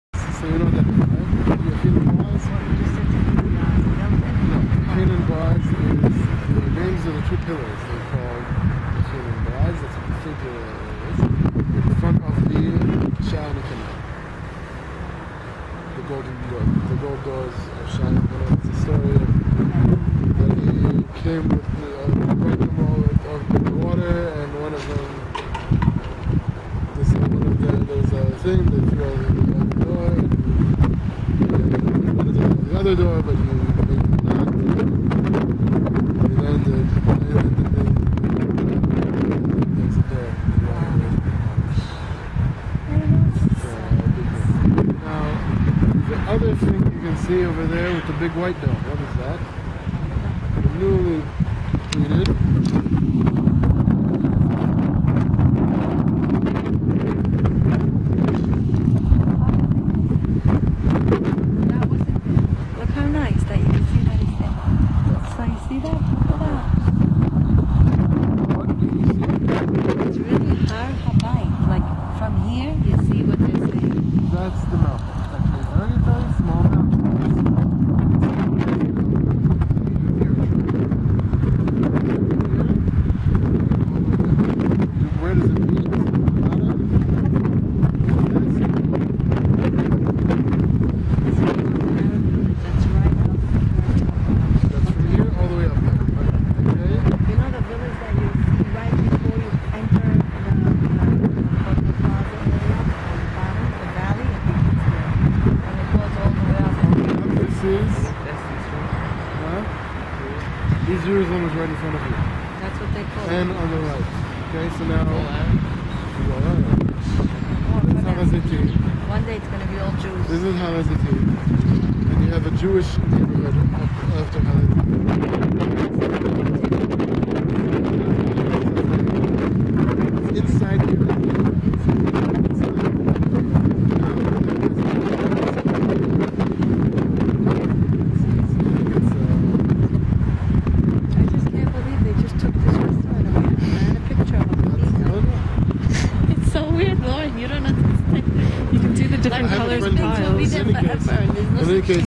Marky explaining landmarks in Jerusalem, Israel
The beautiful landmarks in Jerusalem.